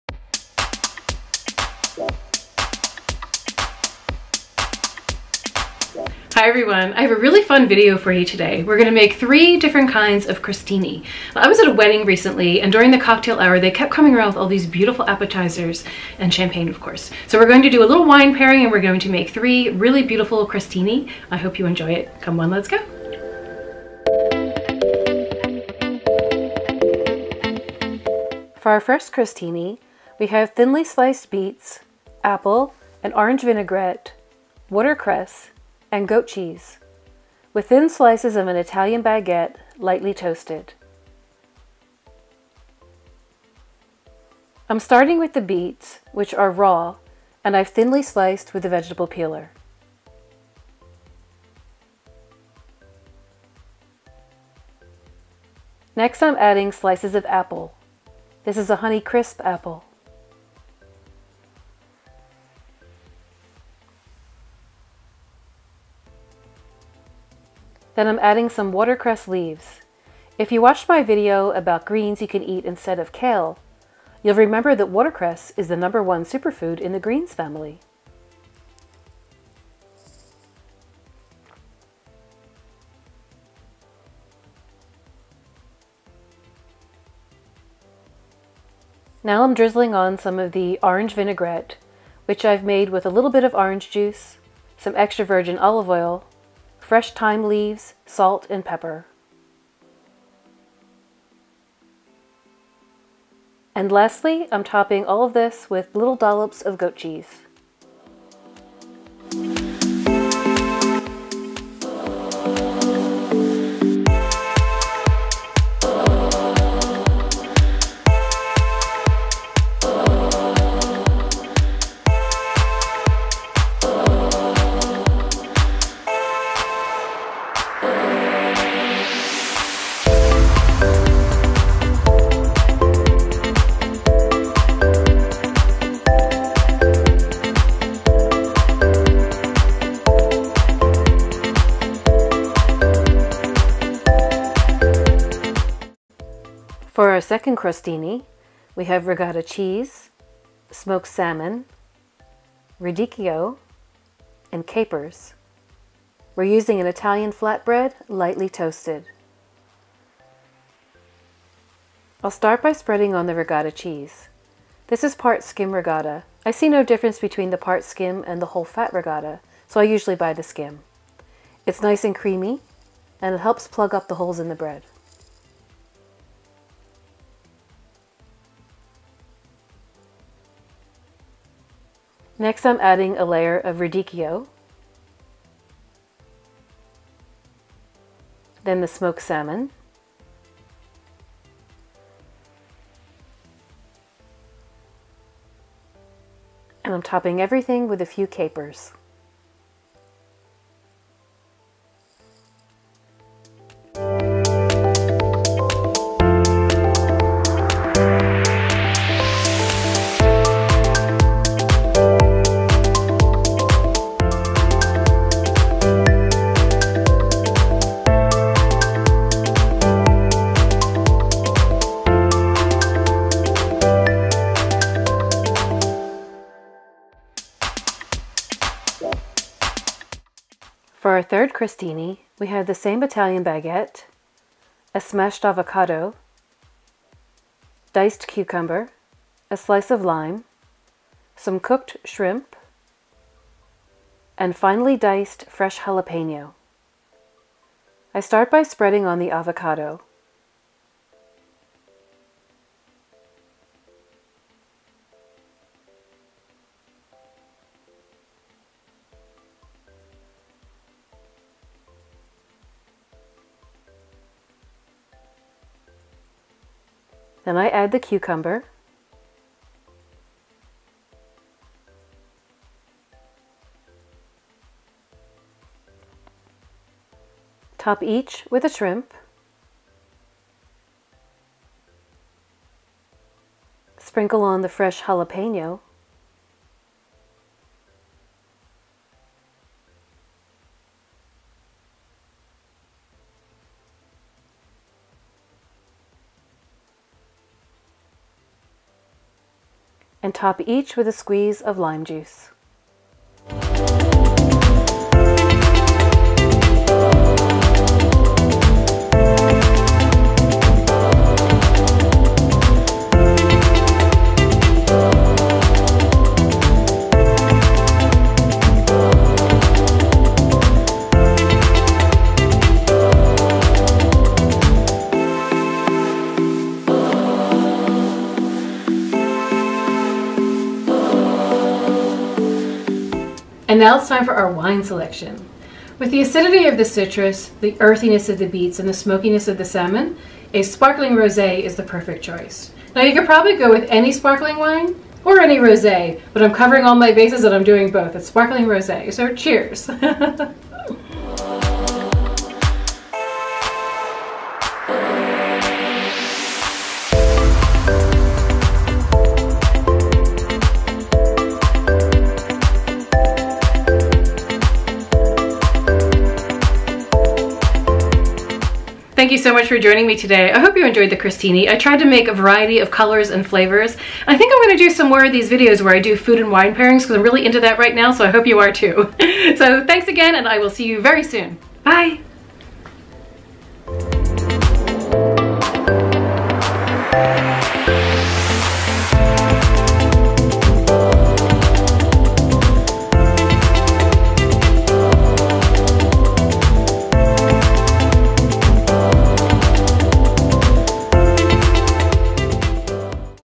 3 Crostini Ideas - Mediterranean Diet Friendly
3 Crostini Ideas - Mediterranean Diet Friendly
Let's make three crostini and select a wine pairing.

The Big Book of Mediterranean Diet Cooking